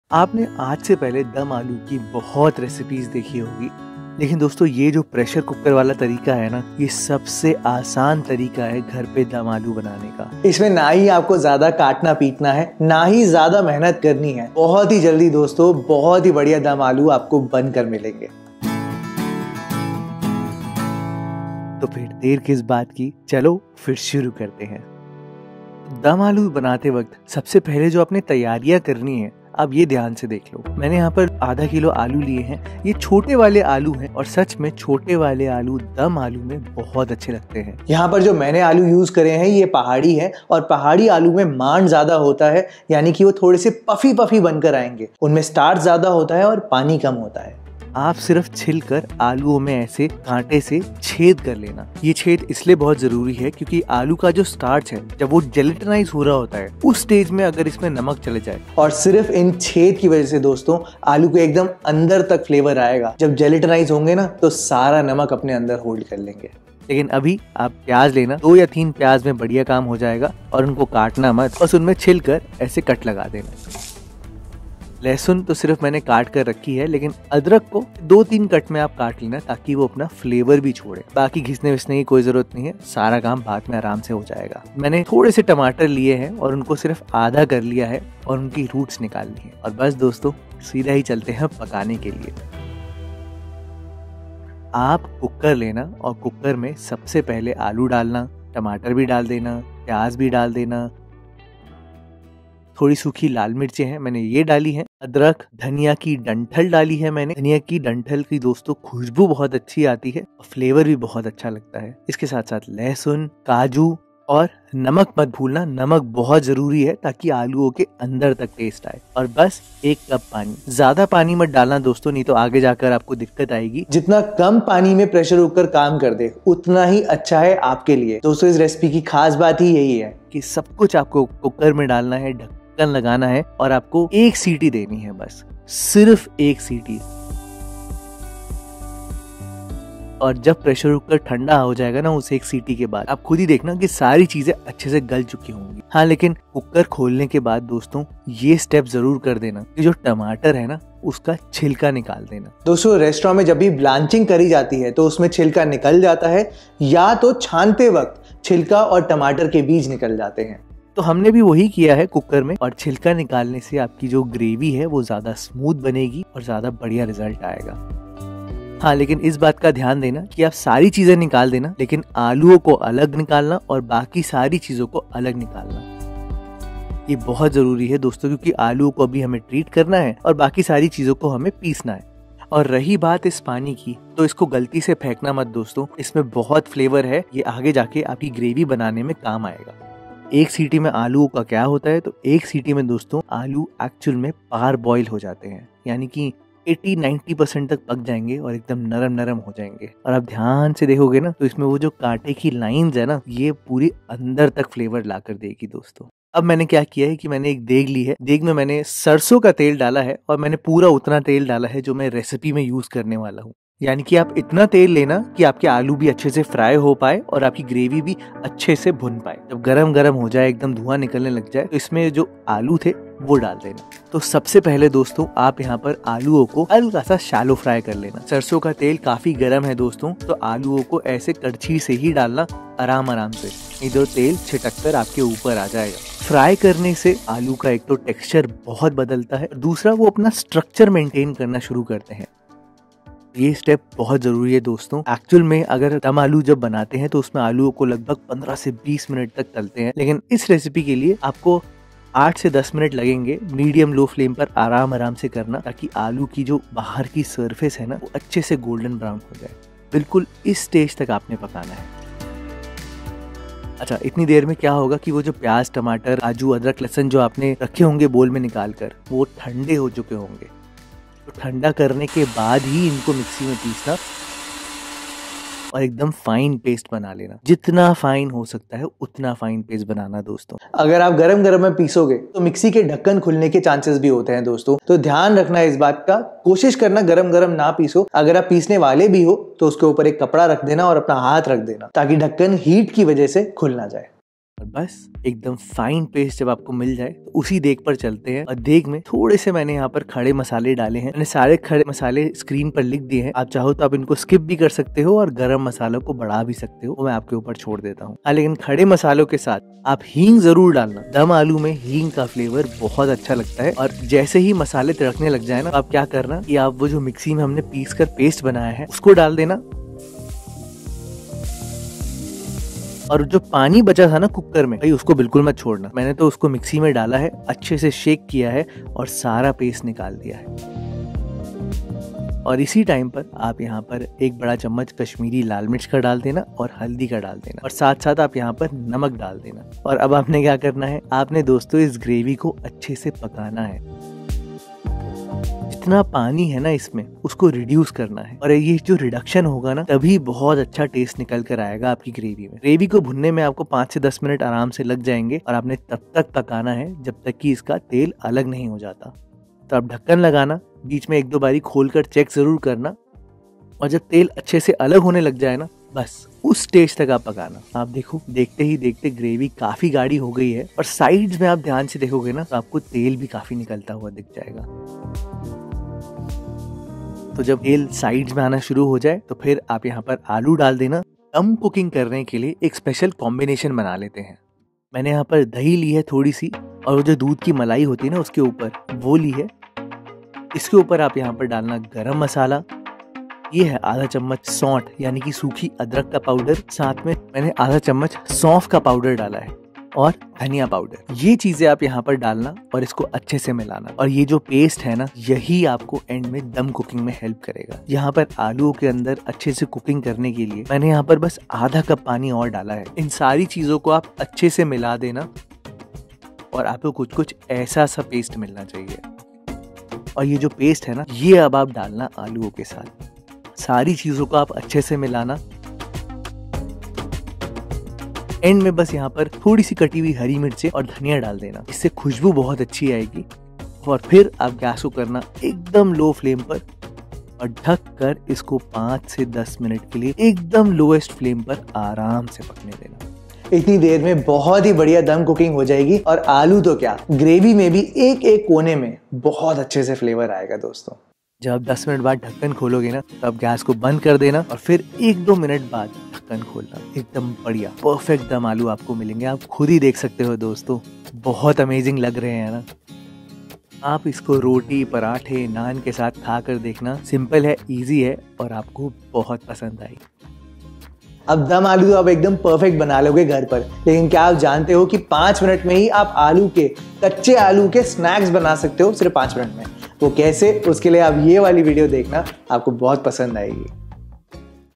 सोचा नहीं था सब कुछ कुकर में डालके के ऐसे दम आलू बनाऊंगा तो इतना टेस्टी बनेगा - Dum ALOO RECIPE
YOUTUBE CREATE
IMOVIE SOUND
BHARATZMUSIC

INGREDIENTS TO MAKE DUM ALOO :
DUM ALOO RECIPE :

500 gms Small Potatoes
2 -3 Onions
8 - 10 Garlic Cloves
2 Inch Ginger
3 Tomatoes
3 Dried Red Chillies
Coriander Stems
10 - 12 Cashews
2 tsp Salt
1 Cup water
3 Laddle Mustard Oil
A Pinch Of Hing
1 Tb
WHOLE SPICES
1 bay Leaf
4 Cloves
1/2 tsp Black Peppercorns
1 tsp Cumin Seeds
4 Green Cardamoms
1 Black Cardamom
1 Cinnamon stick
1 Tbsp Kashmiri Red Chilli Powder
1/2 tsp Haldi Powder
1/2 Cup Curd
1 tsp Garam Masala
1/2 tsp Sauth
2 Tbsp Malai
1/2 tsp Sauf Powder
1 Tbsp Coriander Powder
1/2 Cup Water
3 Green Chillies